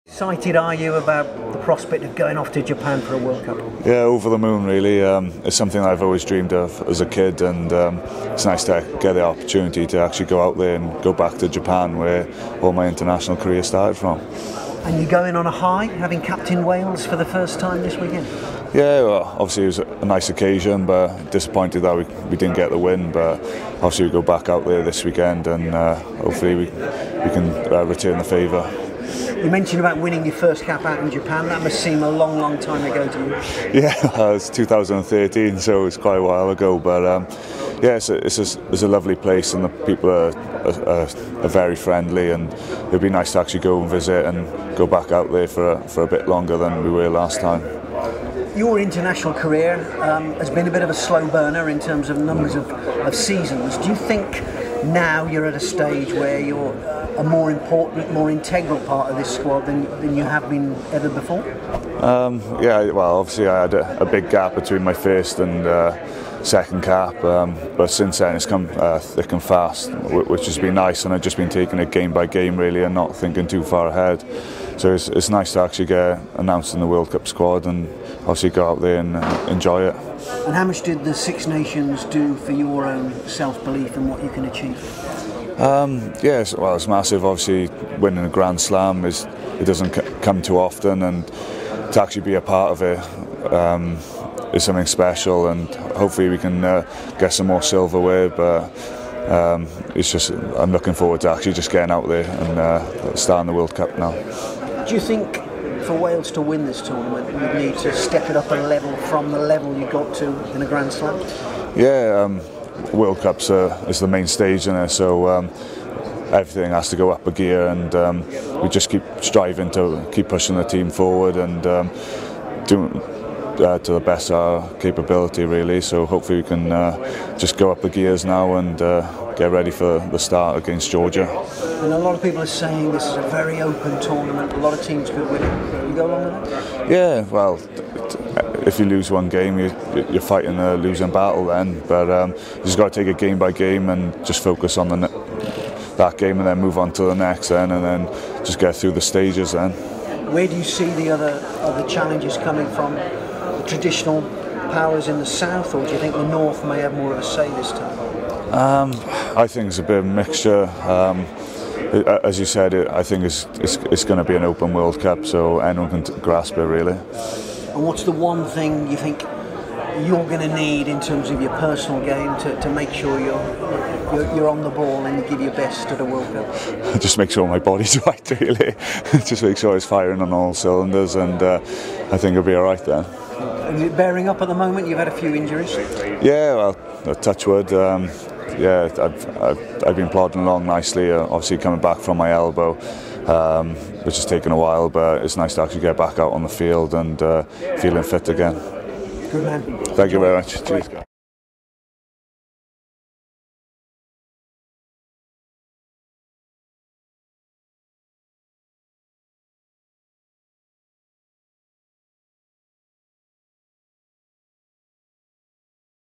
Josh Navidi Looks Ahead To Rugby World Cup
Back-rower Josh Navidi speaking at Wales squad media session ahead of 2019 World Cup in Japan.